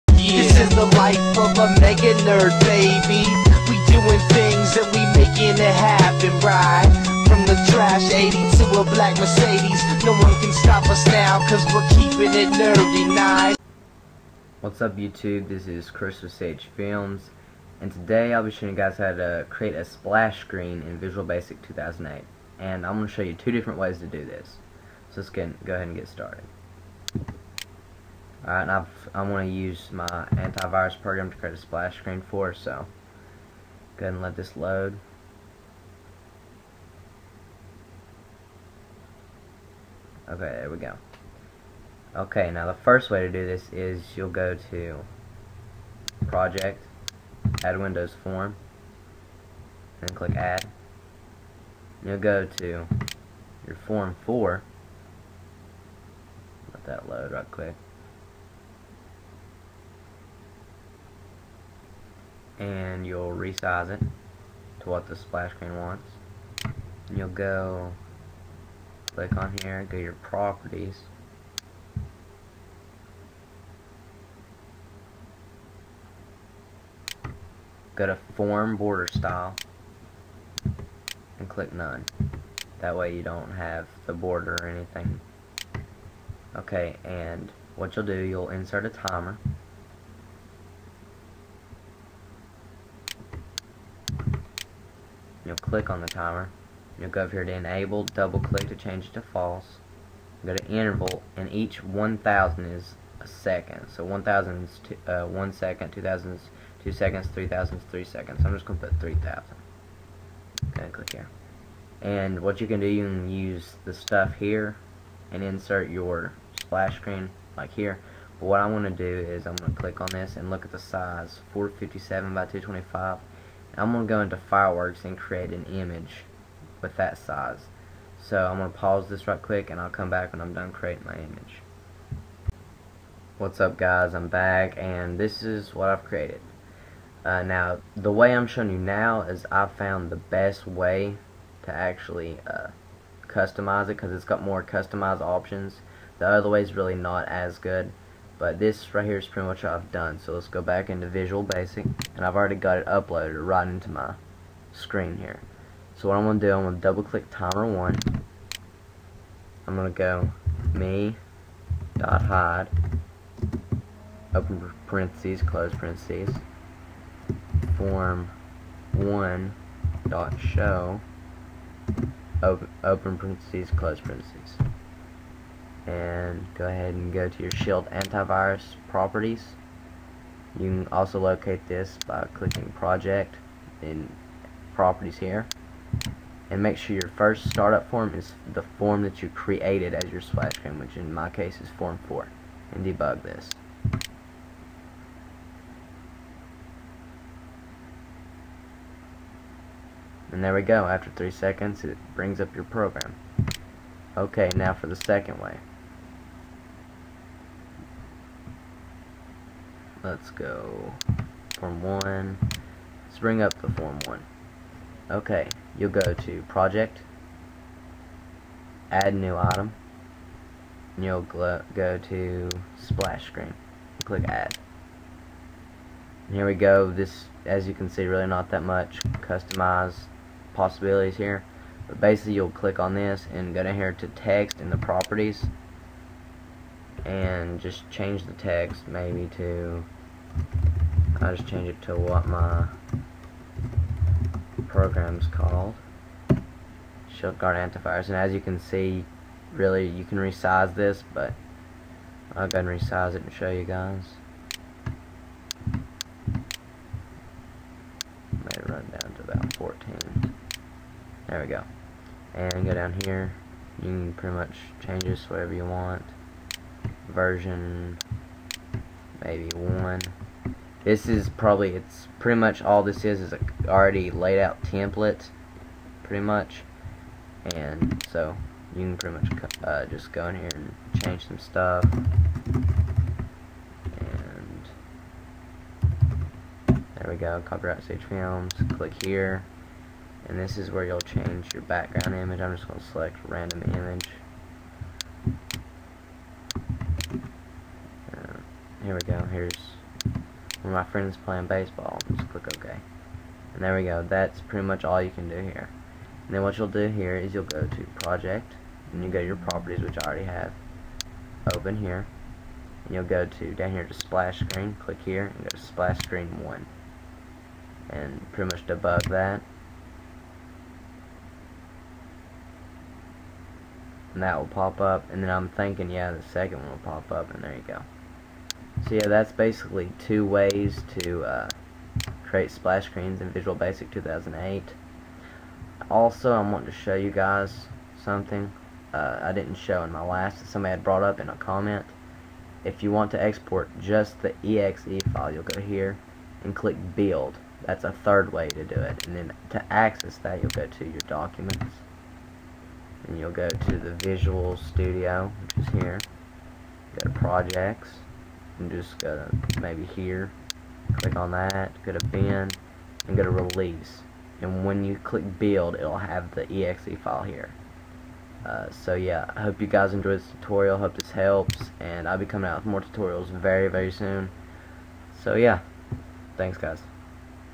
Custom Splash Screen - VB 2008
This Tutorial Shows You How To Create A Custom Splash Screen in Visual Basic 2008. Also I Will Show You Another Way To Export A Program Into .exe File. Enjoy. Rate 5*.

Timer:
Me.Hide ()
Form1.Show ()